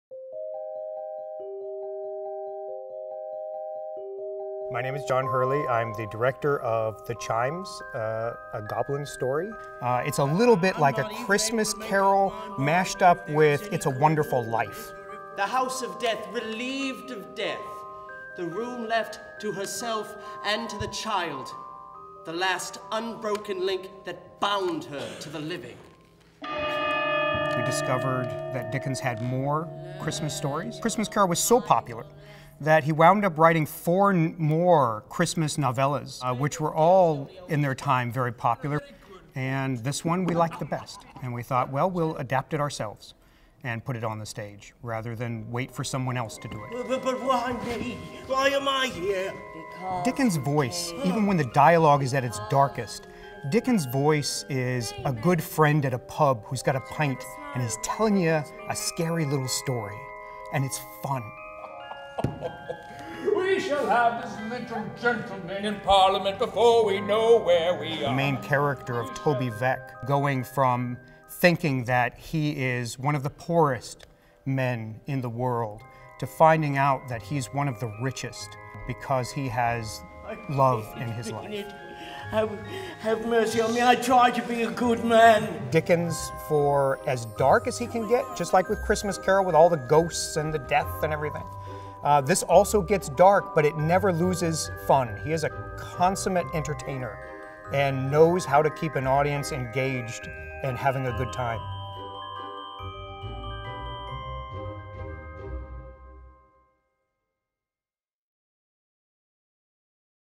THE CHIMES: A Goblin Story Trailer (1min 50 sec)
December 2 - 18 at the Road Less Traveled Theater

Published one year after A Christmas Carol, Charles Dickens’ little known Holiday tale is brought to life in this world-premiere adaptation in which five actors play nineteen characters in a fast-paced, warm-hearted fable of fun and fright. It’s New Year’s Eve and old Toby Veck and his daughter Meg have little in life except each other's love. But when the ancient church bells chime at midnight, the Goblins of the Bells will teach poor Toby a frightening lesson of what it is to be rich in life—by taking his away. Starring Andrea Andolina, Gerry Maher, Lisa Vitrano, Nick Lama and Steve Brachmann.